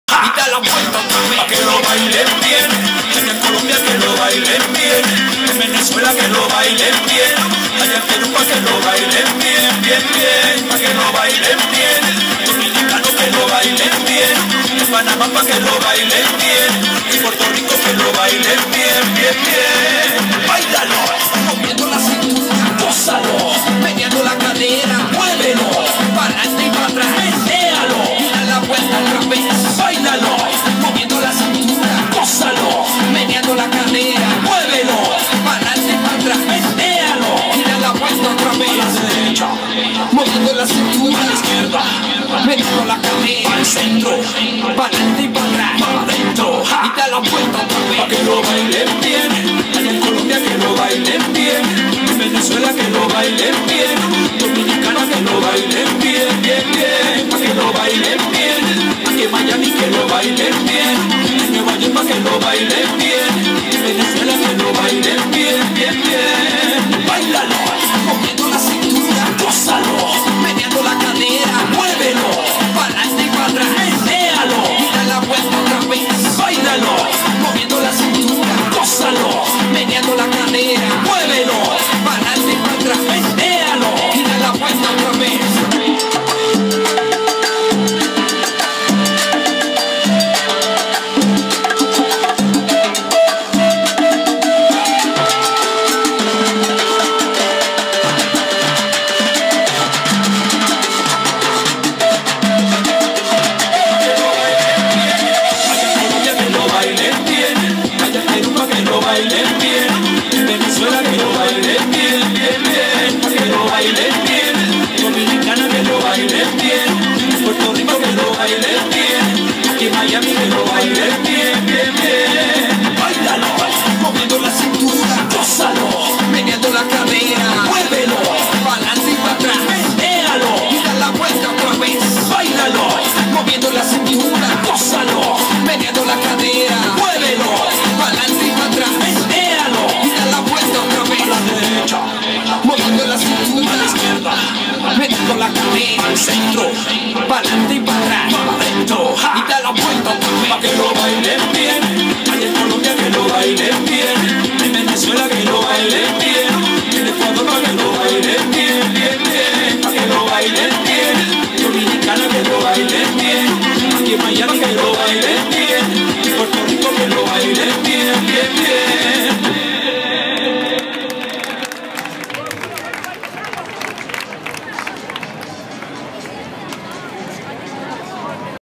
Zumba piluca
Plaza del pilar día día del deporte 22 de septiembre